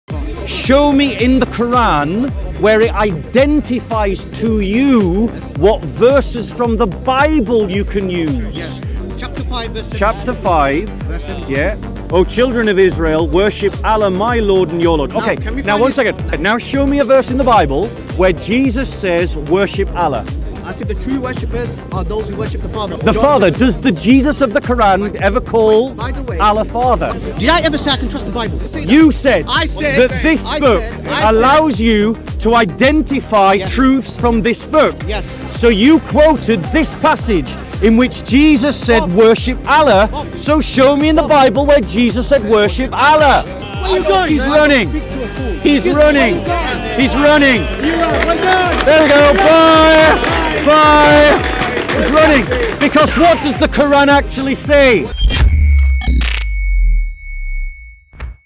Does the Jesus of the Qur'an ever call God "Father" | Bob | Speakers' Corner
:::::: Topic Highlights:::::: Another Dawah script fail
 | Speakers' Corner | London UK
Speaker:  @bobofspeakerscorner1099
Shot & Edited:  @SocoFilms
Post:  @socostudio
:::::::::IF YOU WOULD LIKE TO SUPPORT US ::::::
👉 Become SOCO Members to have early access to new videos: 👇👇👇